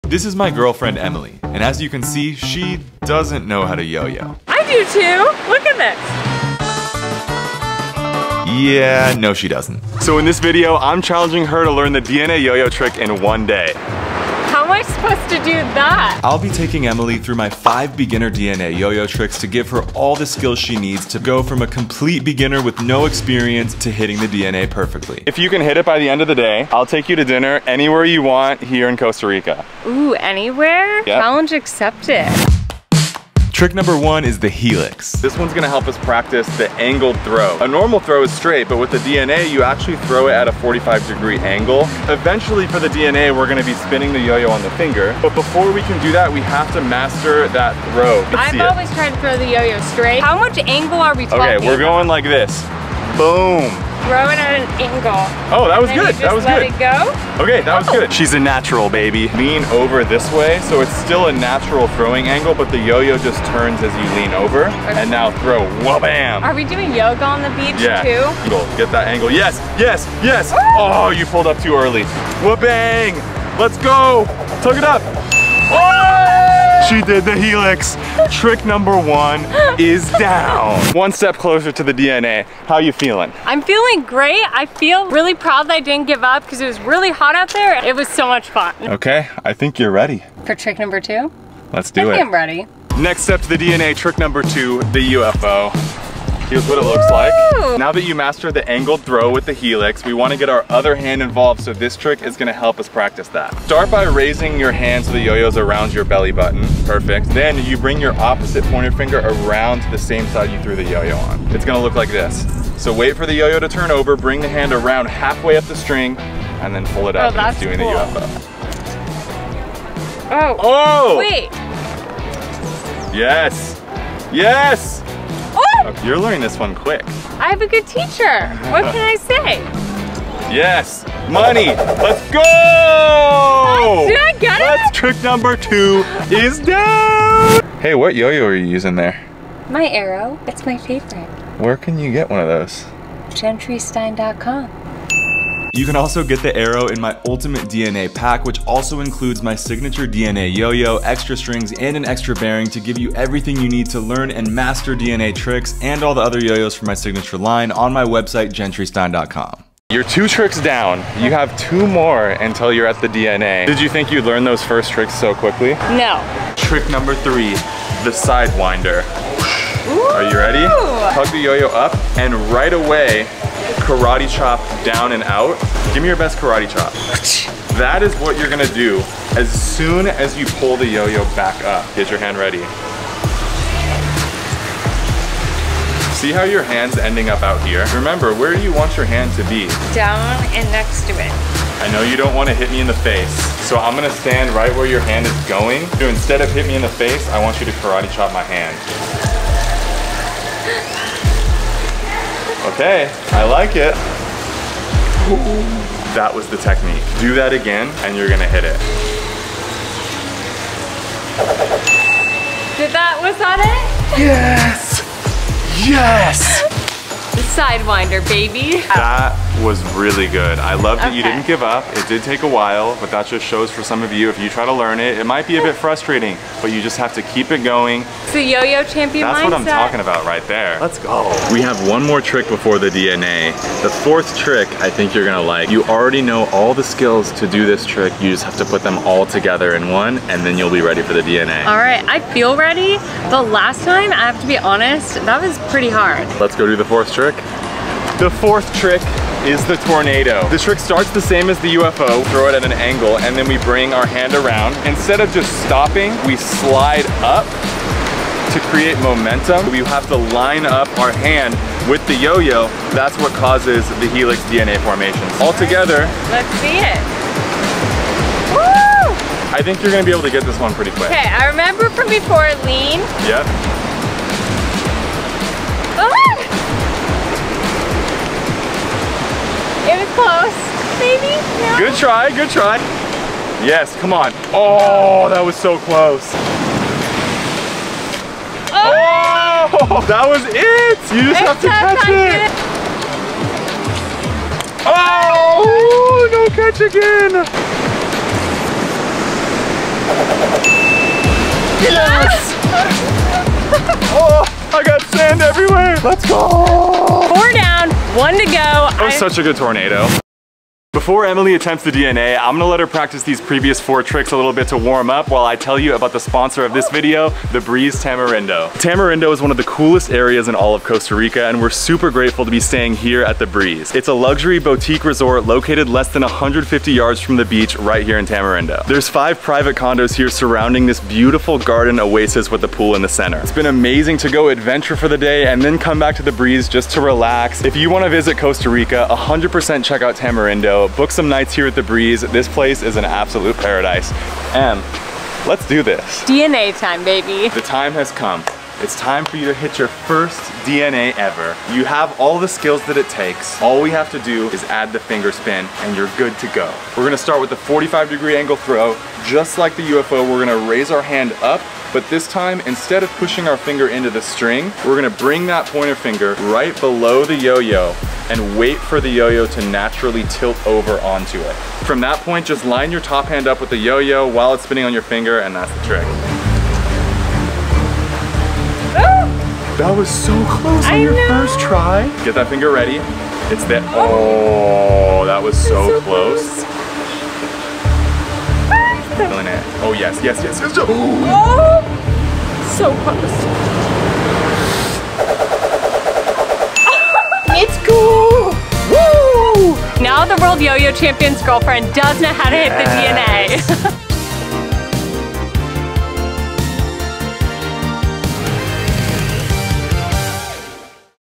Can My Girlfriend Do The DNA Yoyo Trick?
I challenged my girlfriend to do learn how to do the DNA yoyo trick in 1 day! Watch to see if she did it!

Book a stay at The Breeze in Tamarindo Costa Rica